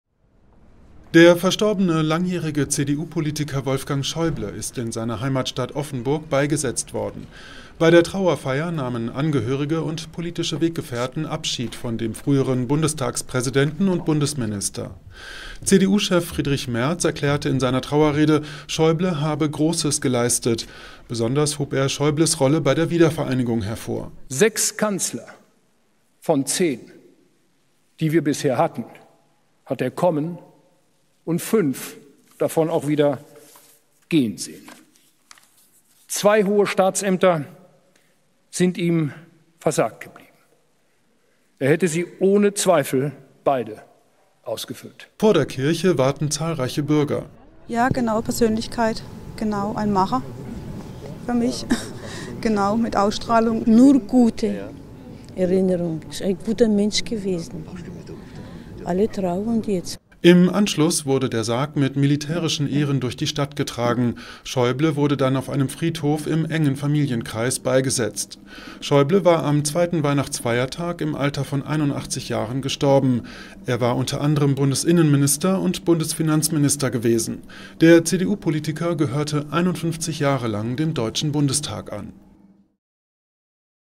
Abschied von Wolfgang Schäuble in Offenburg | AFP
Bei einer Trauerfeier in Offenburg haben Angehörige und politische Weggefährten Abschied vom verstorbenen CDU-Politiker Wolfgang Schäuble genommen. Anschließend wurde Schäuble im engen Familienkreis beigesetzt.